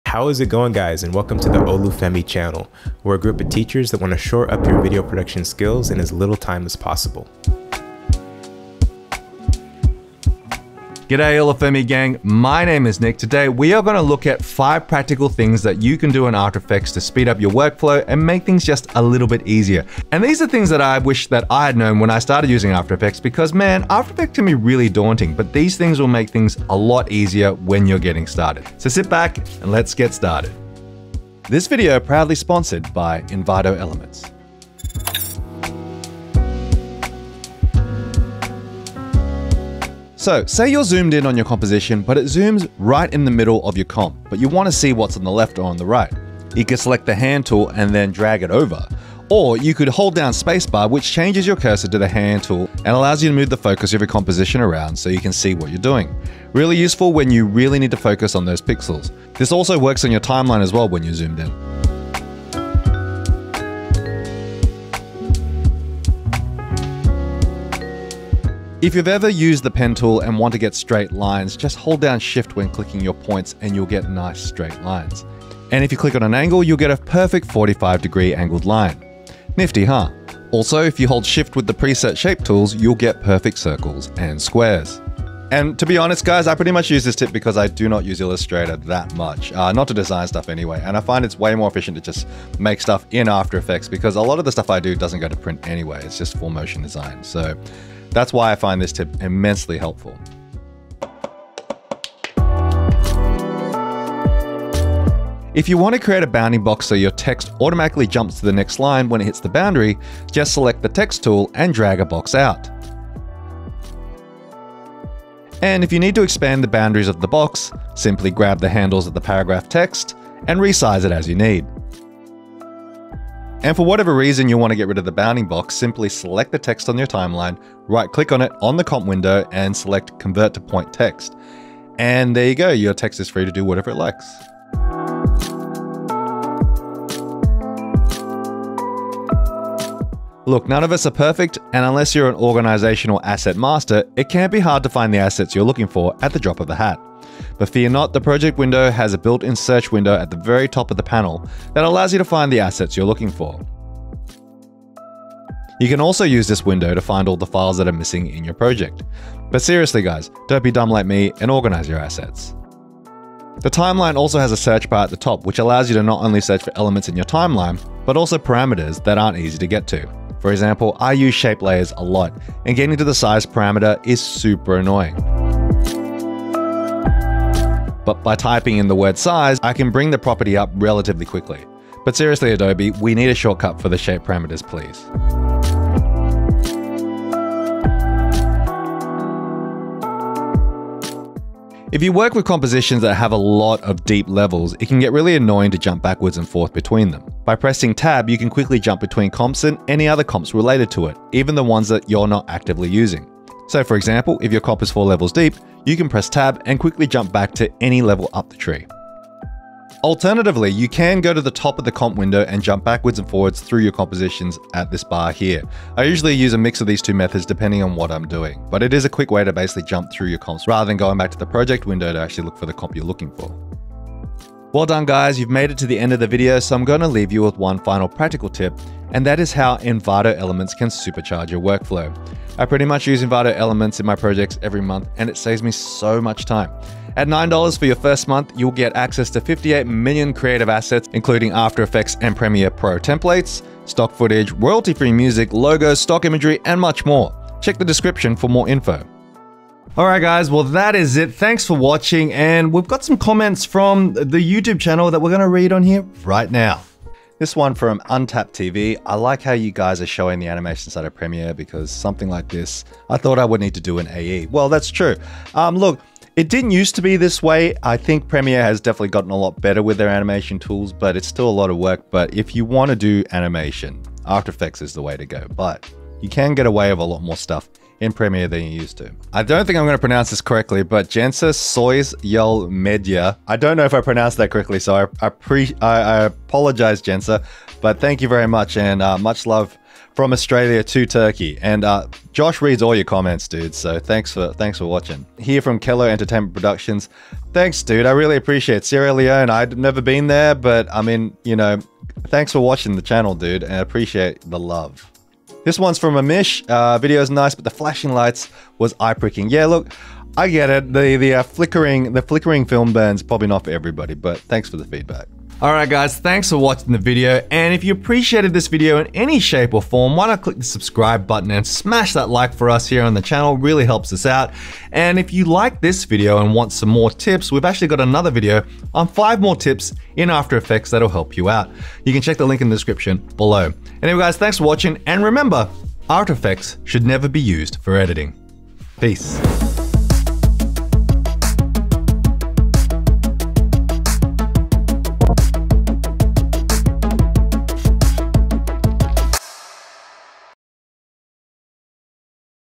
5 After Effects Tips I Wish I Knew Sooner
You should know these After Effects Tips and Tricks. They will ave you A LOT of time in AE!